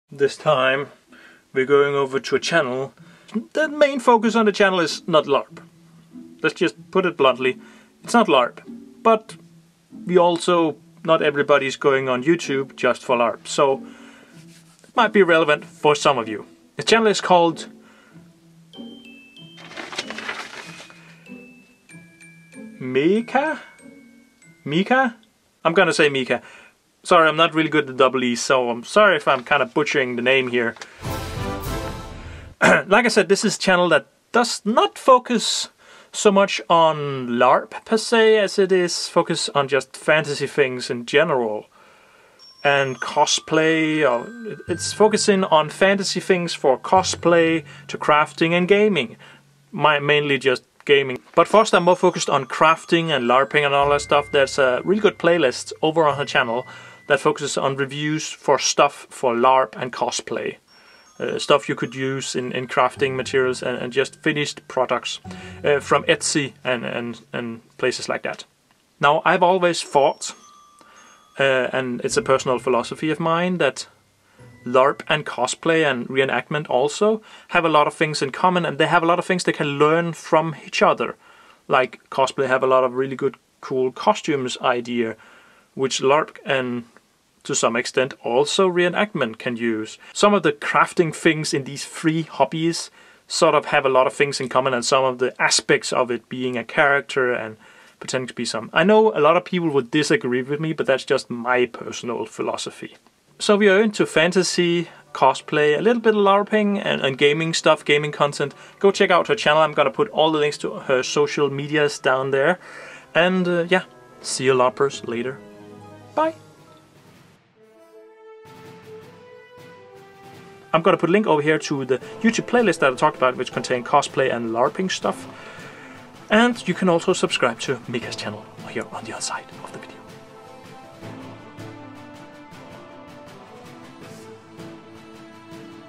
Meeka - LARPer ShoutOuts
Not all channels for the LARPerShoutOut is only for LARP.
Meeka is a channel with multiply types of videos.
But what is especially interesting for us LARPers are the Cosplay and LARP playlist.

- the LarpForge team